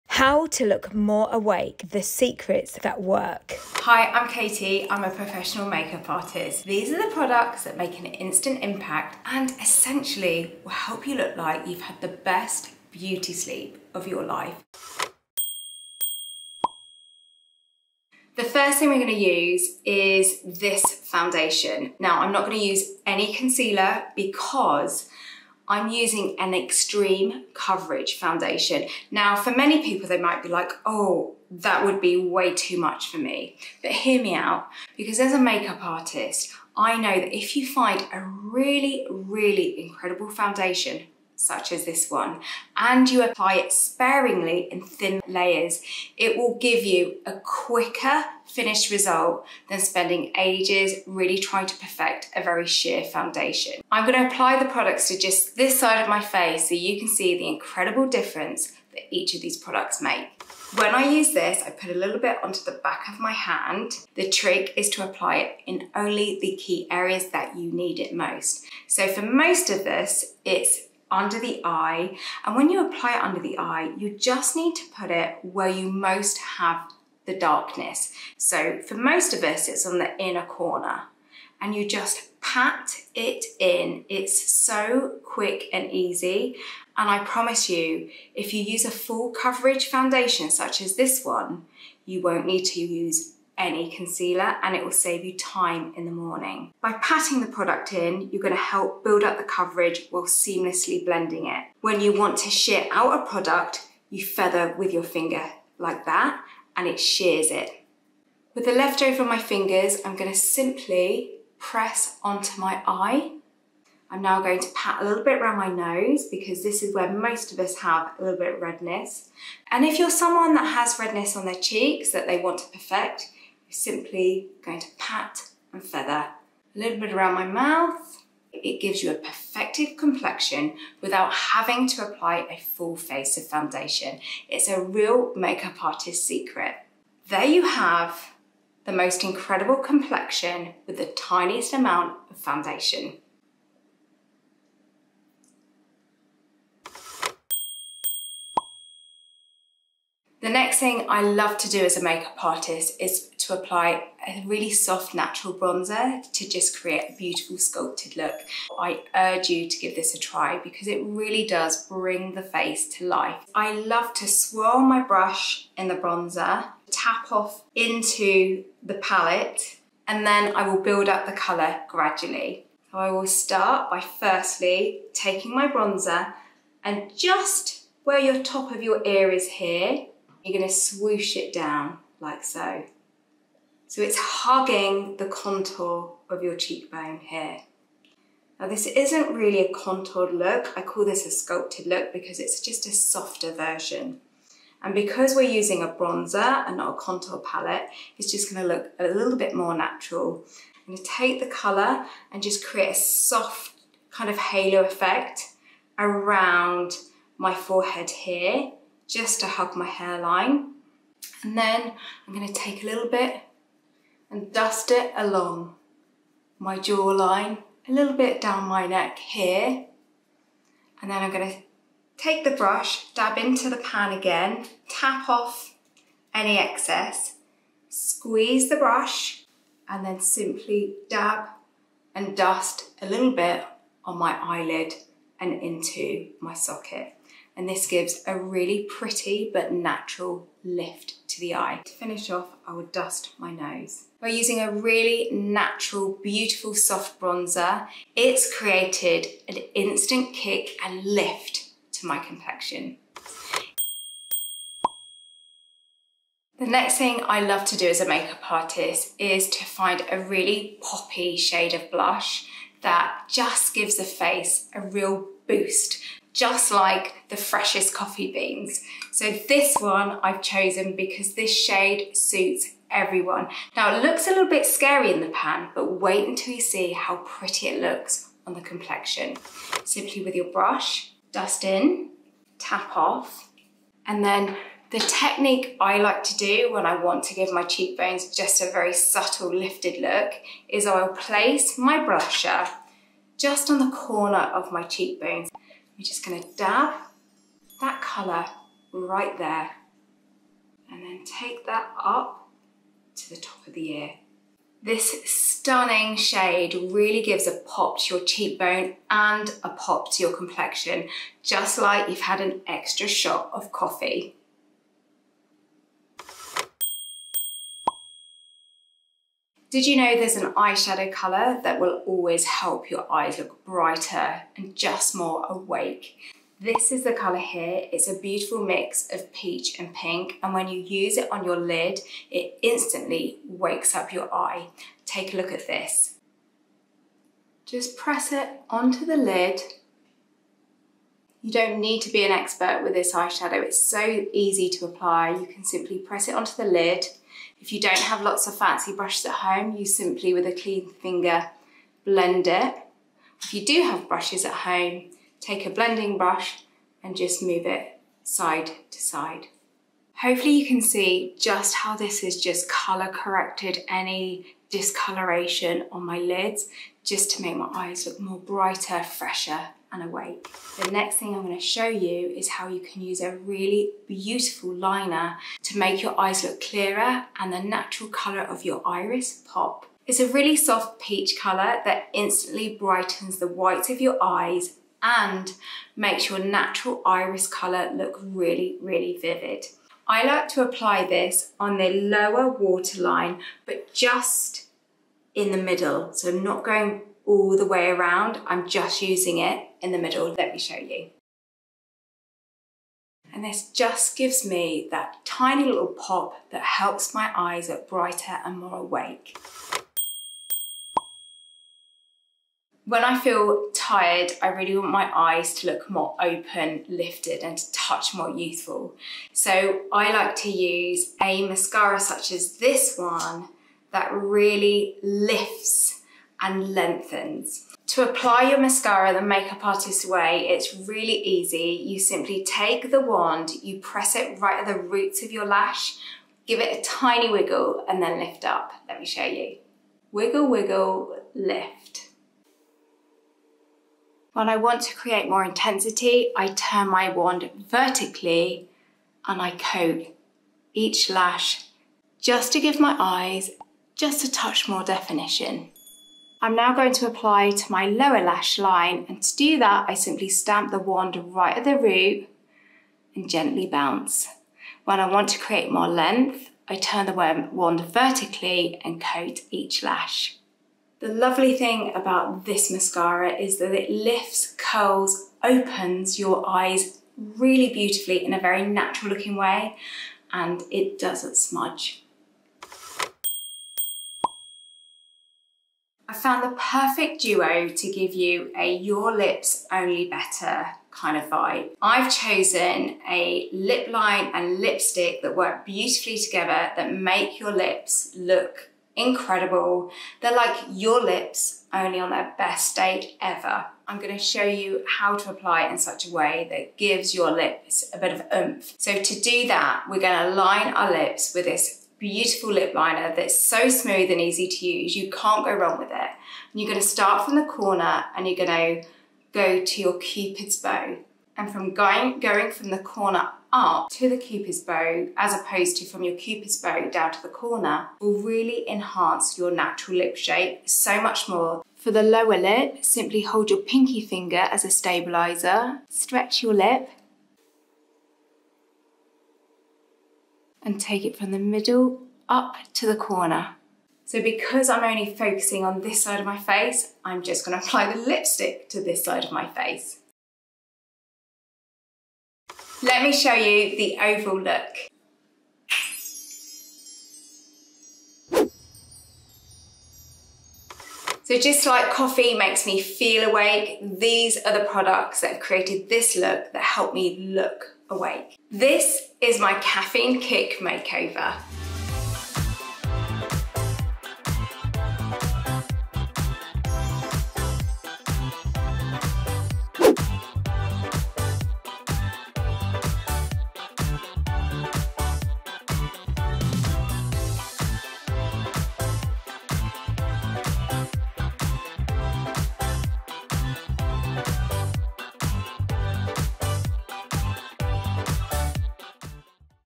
HOW TO LOOK LESS TIRED! ☕️
🙋‍♀️Hi Friends, Makeup Artist Katie here!

Just like my morning coffee helps me feel more awake...

...I'm always on the hunt for makeup that helps me look more awake & rested, especially on those tired, lacklustre days 😴

🎬 Watch my video to discover 6 makeup products that give your face an instant lift & boost when your skin needs a little pick-me-up…

…like a shot of espresso - but for your complexion! ☕️

Love, Katie 💖✨
Makeup Artist & Founder of

🙋‍♀️P.S. What tutorial would you like me to make you next?

THE SMALL PRINT: I have not been asked to or paid to make this video.